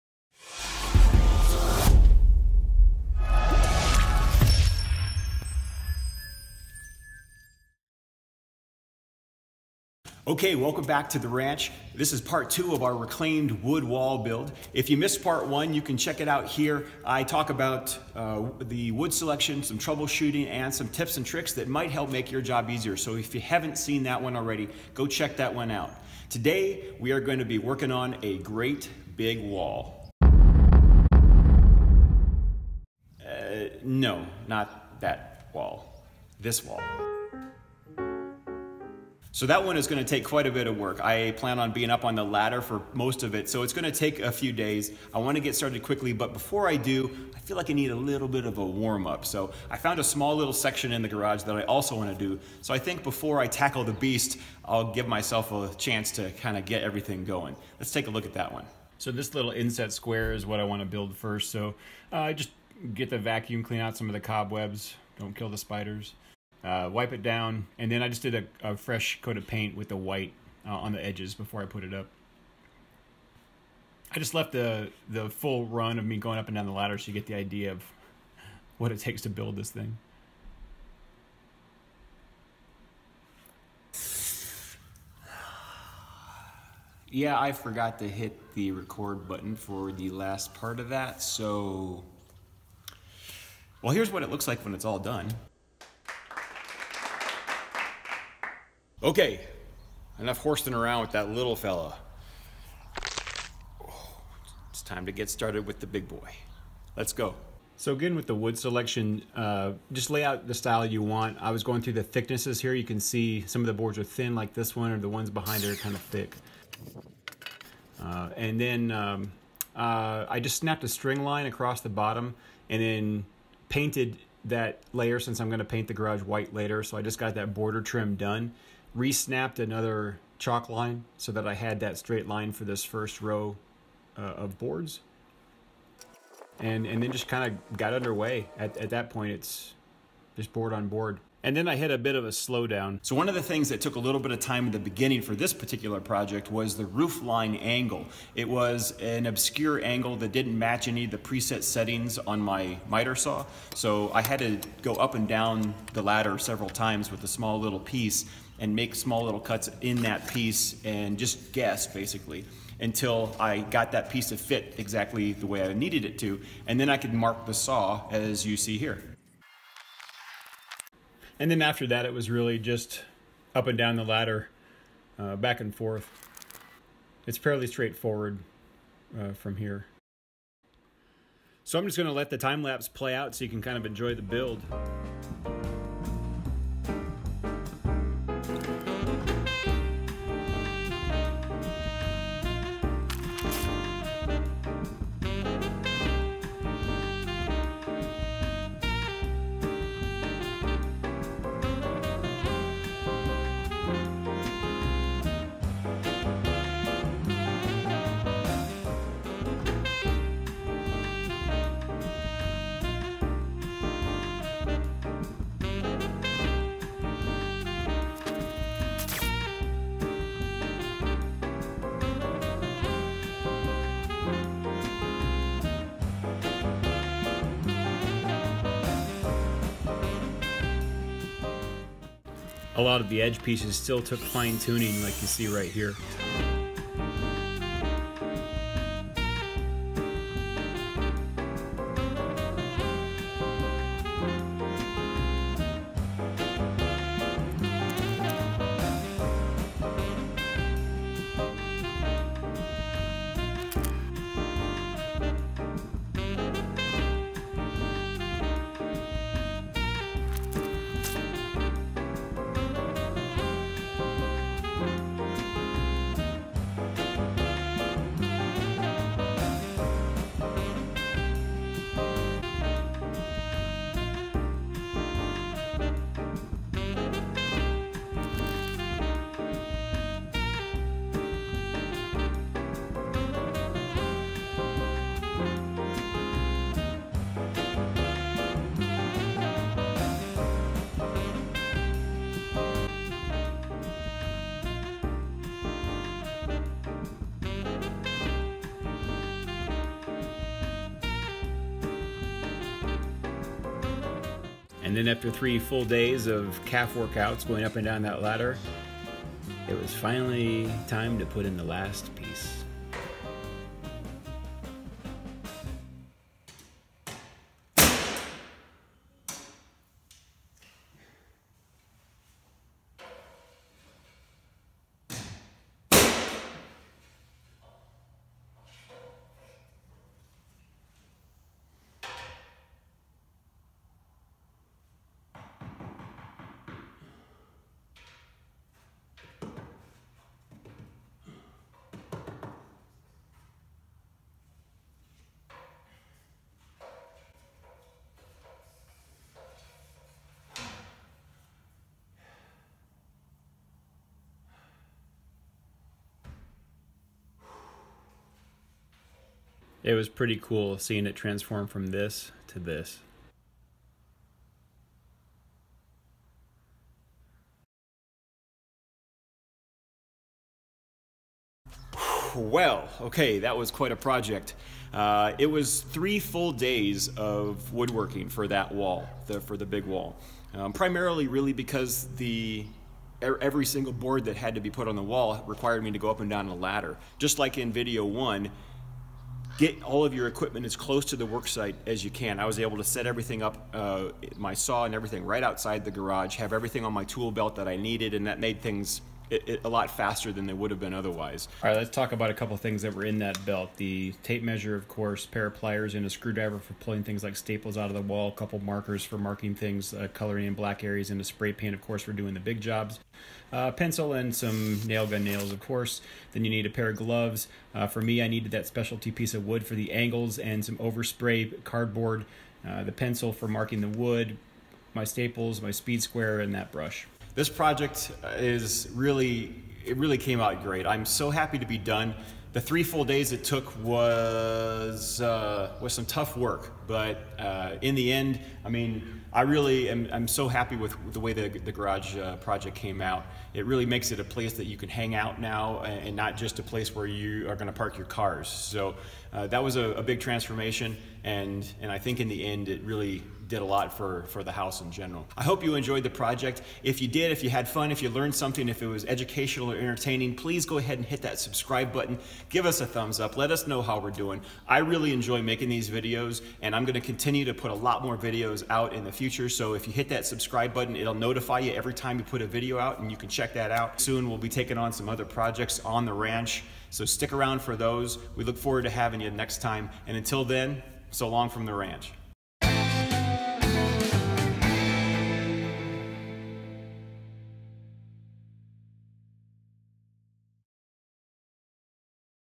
Full DIY Big Reclaimed Wood Wall Build...For FREE! Pt 2/2
In part 2 of our reclaimed wood wall builds, we'll tackle The Beast!  A big section of the garage wall is due for an upgrade.  Can I do it?  How long will it take?